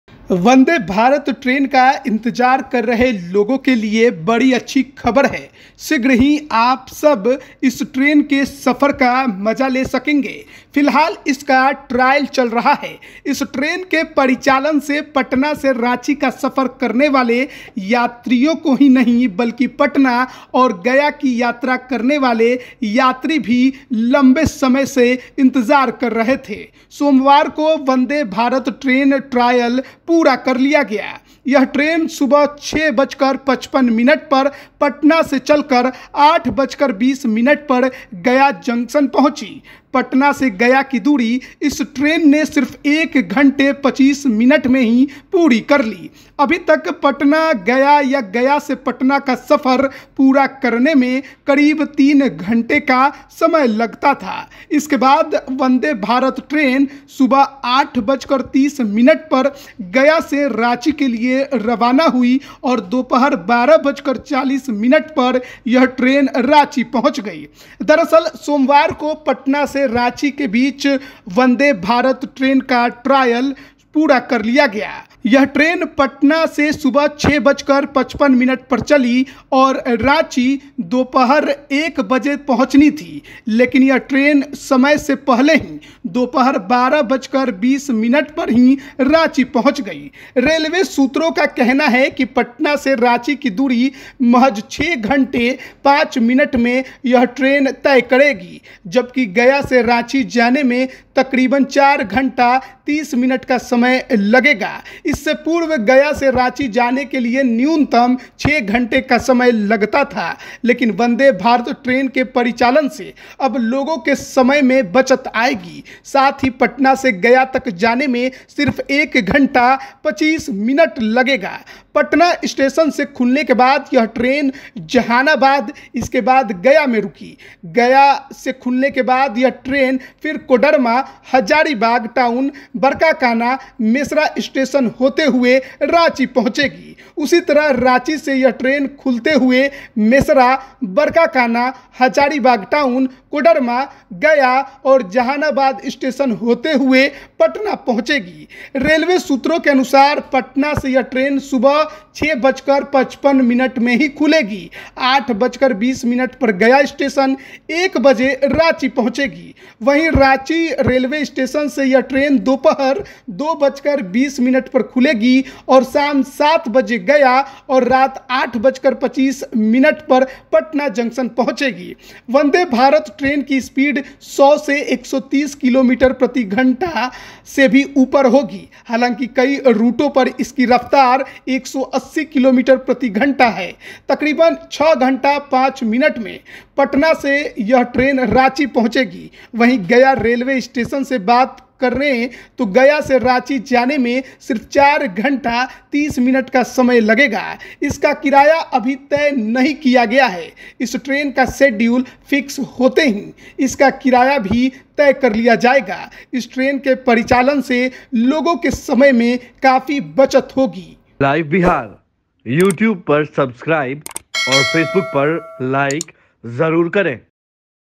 Patna to Gaya और रफ्तार में Ranchi, Vande Bharat से कम समय में सफर सबसे आसान, जान लीजिये
Live Bihar Digital Network